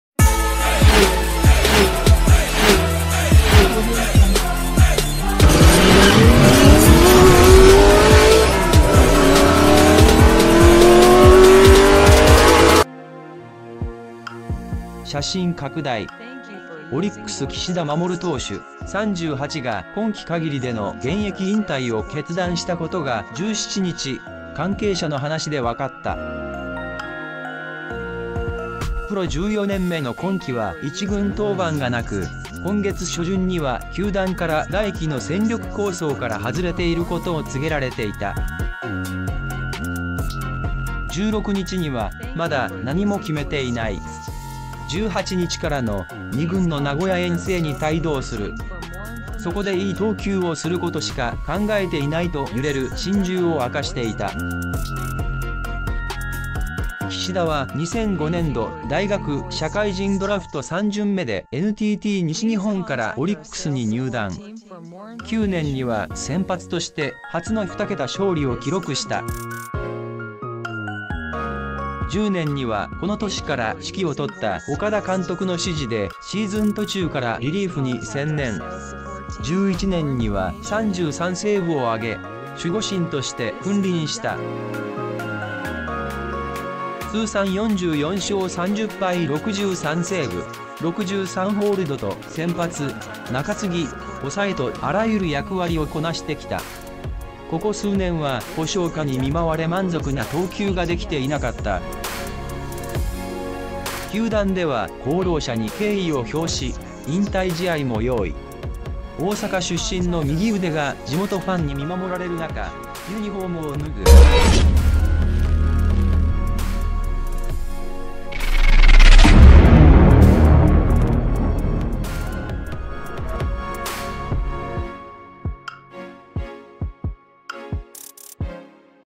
オリックス岸田護が引退 戦力構想外を告げられ決断 ０９年に２桁星、１１年３３ＳTop News
オリックス岸田護が引退 戦力構想外を告げられ決断 ０９年に２桁星、１１年３３ＳTop News Thanks you verry much, like and sub video channel:► Subscribe for more 写真拡大